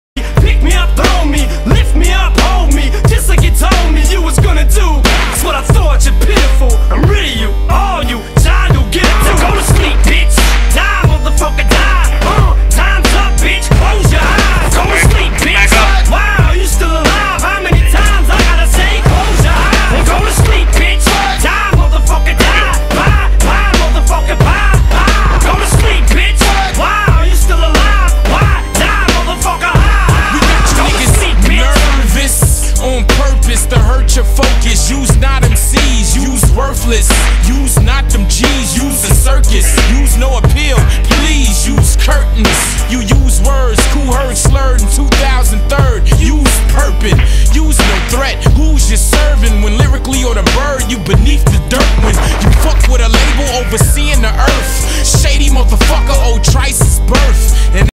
Epic Boxing Match (Cliff vs Jake)
Cliff vs Jake in an epic basement cage fight